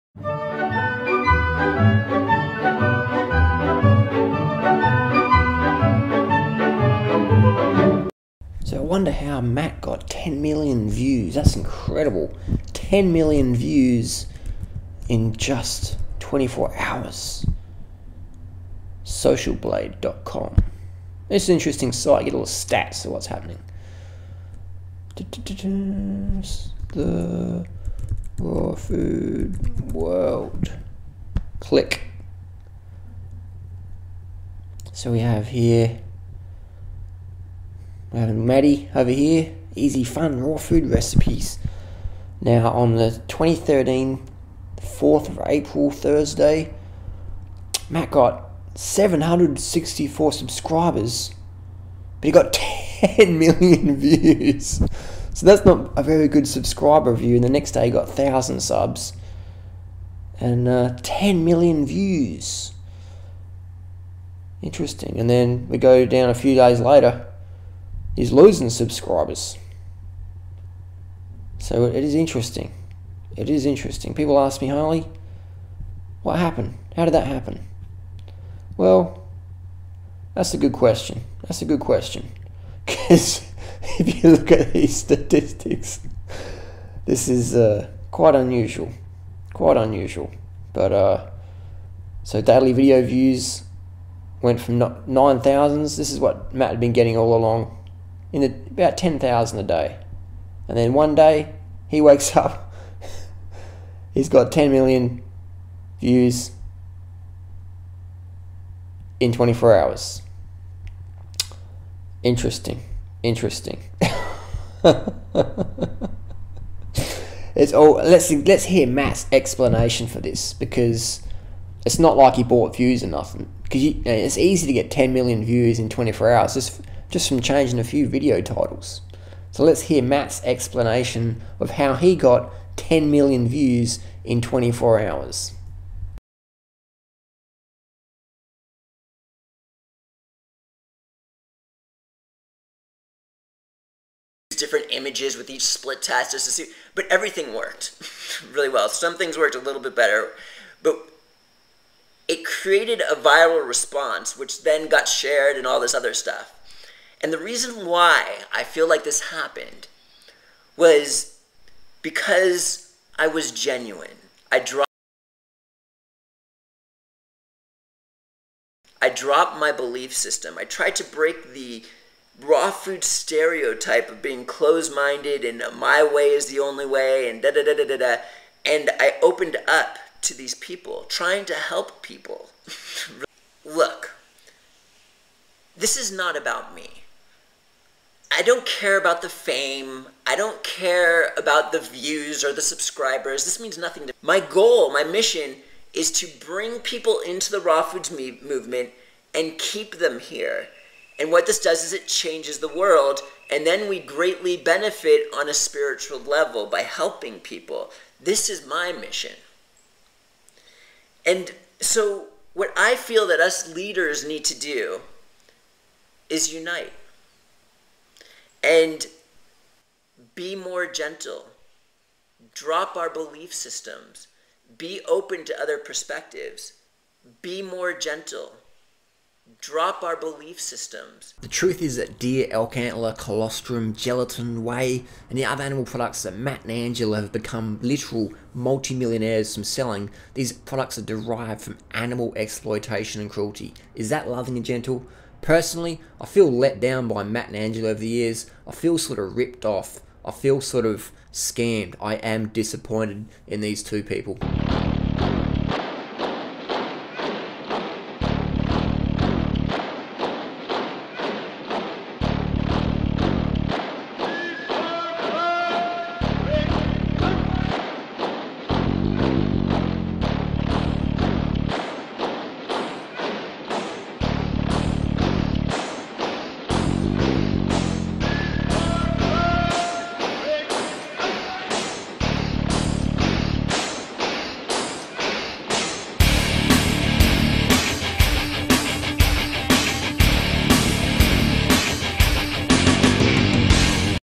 Matt & Angela Buy 10 Million Views Or Was It Legit like they said it was
Matt & Angela Buy 10 Million Views Or Was It Legit like they said it was? Nothing personal. Just sharing my comments and criticisms. Matt & Angela said they got '10 million views because they became more open and loving'. Interesting. Was it due to purchasing Black Hat SEO views?